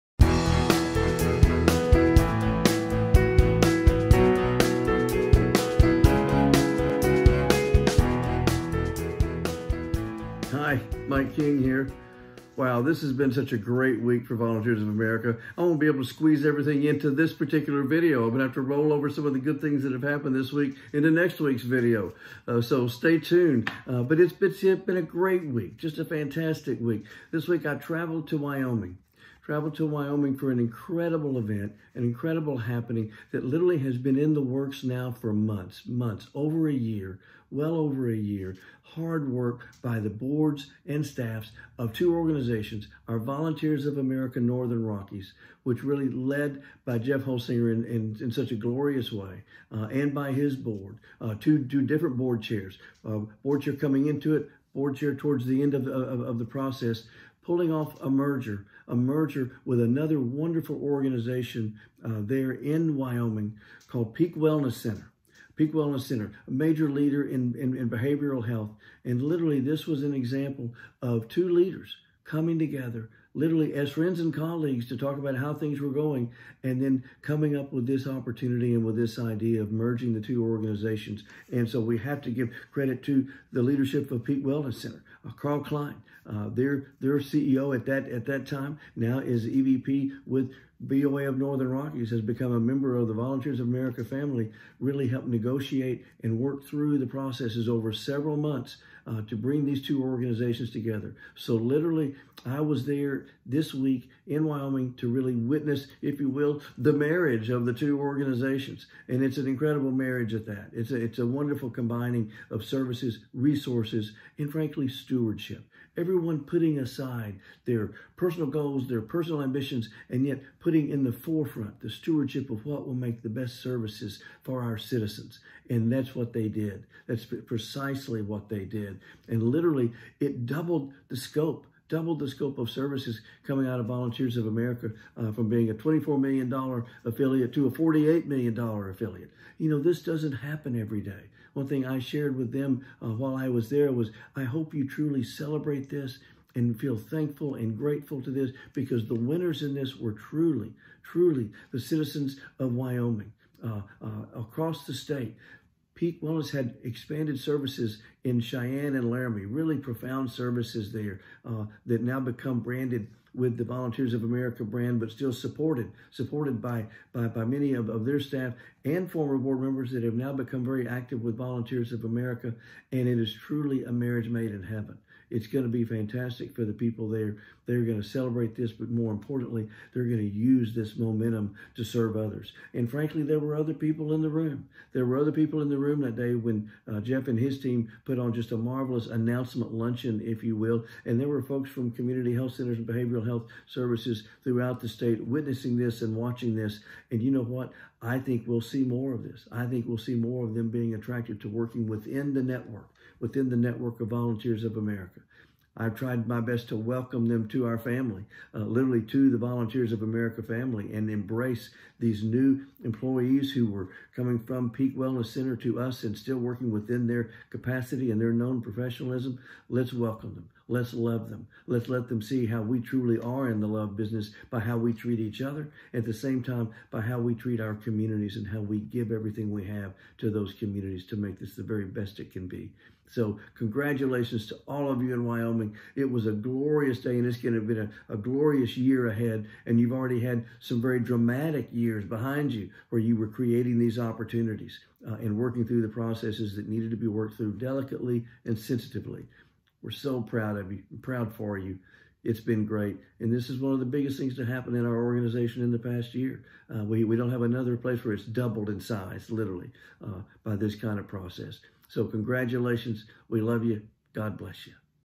A video message from our CEO Mike King | 09_10_2021
Mike commends the merger between VOA Northern Rockies and Peak Wellness Center in Wyoming.